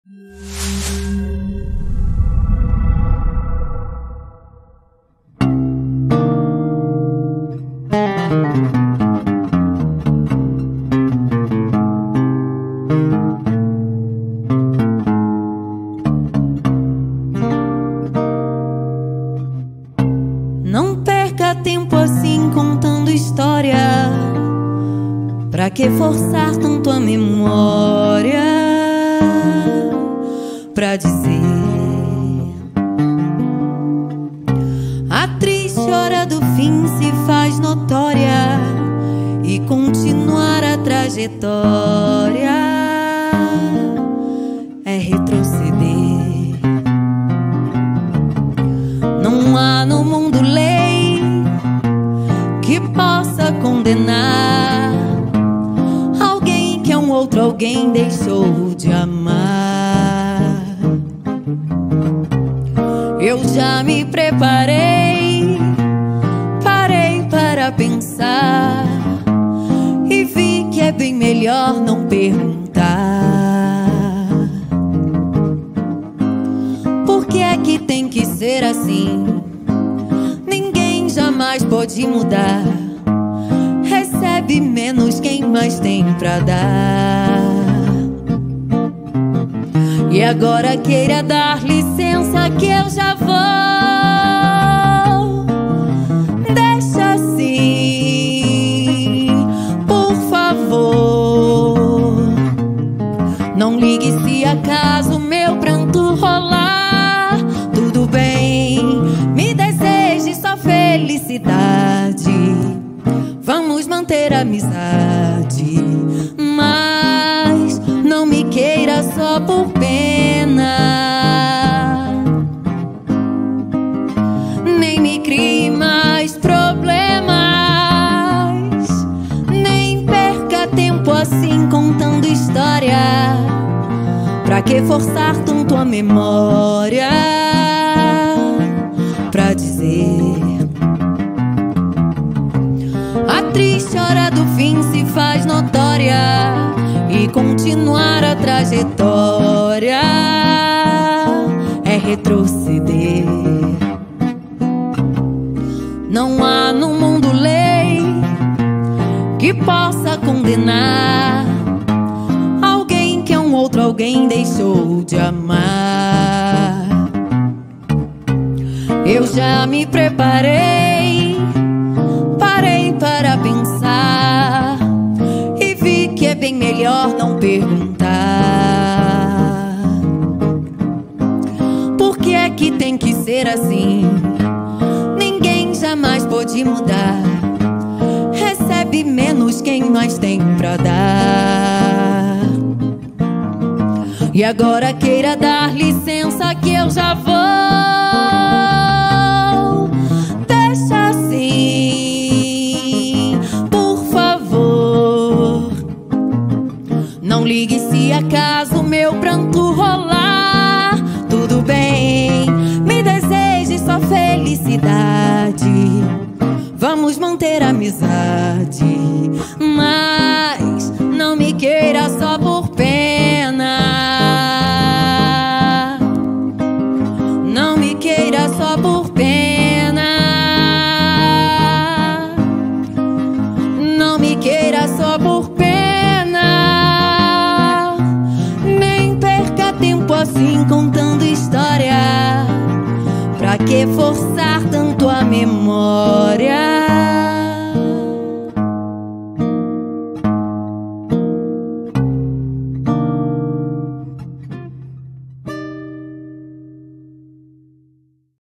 Talita Duarte - Trajetória
Artista: Talita Duarte

Conheça Talita Duarte, a cantora que nasceu com música nas veias!

Desde pequena, ela encantava a todos em festas de família com sua voz melodiosa. Ao crescer, ela decidiu aprimorar sua técnica e se juntou ao Coral da Escola de música, onde começou a aprender sobre harmonia e ritmo.

E agora, morando em São Paulo, ela está espalhando todo o seu talento pelas noites paulistanas, levando o swing e a alegria do samba carioca para todos os cantos da cidade.

Se você ainda não teve a oportunidade de assistir a um show da Talita, não sabe o que está perdendo!

Venha conhecer essa artista incrível e se deixe envolver pela sua música e sua beleza deslumbrante!